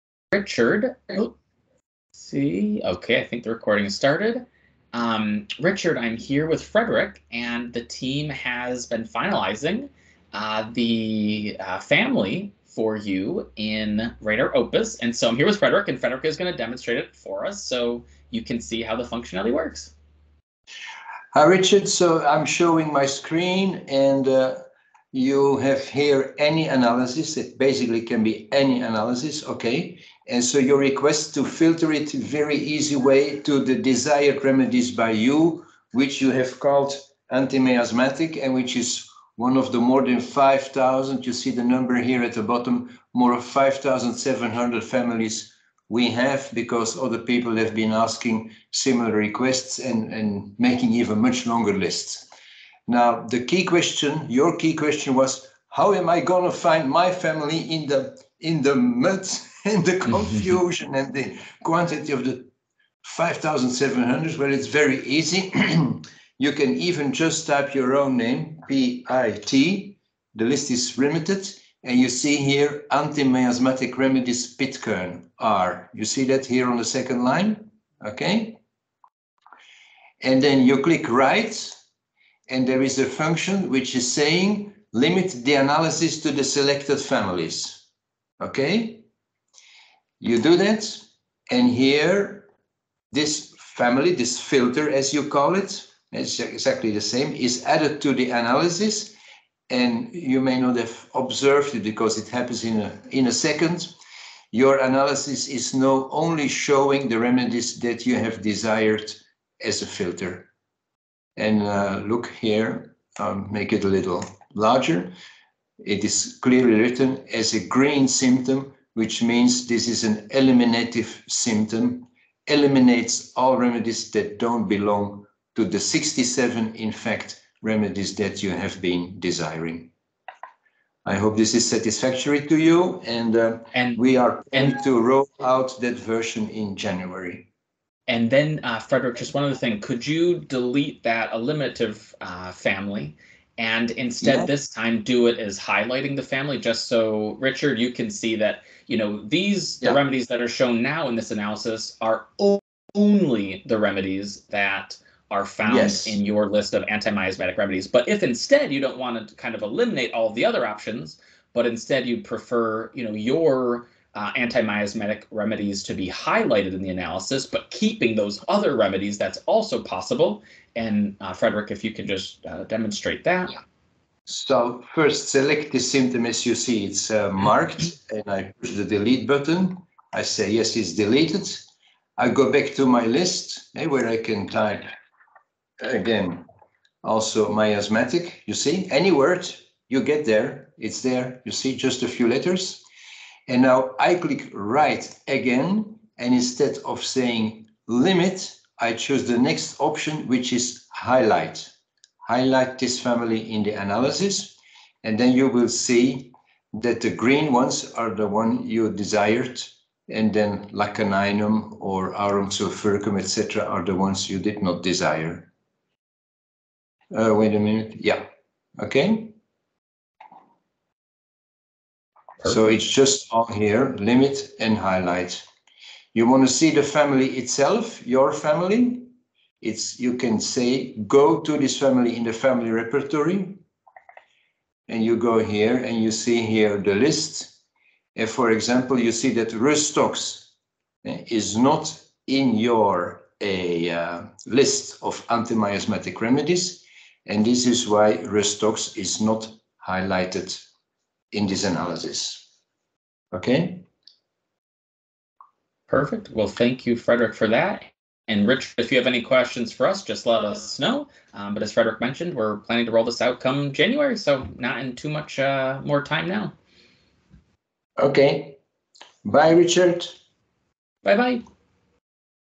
Presenting Pitcairn's Anti miamsatic filter
Richard Pitcairn (USA) has collected a set of anti-miasmatic remedies from Hahnemann, Bönninghausen and Boger.
He likes to apply these remedies as a filter so only these are shown in the analysis.
Frederik Schroyens and Timothy Miller present this filter (called “family” in RadarOpus) and how it can be used.
This feature is included in RadarOpus 3.3 and later versions.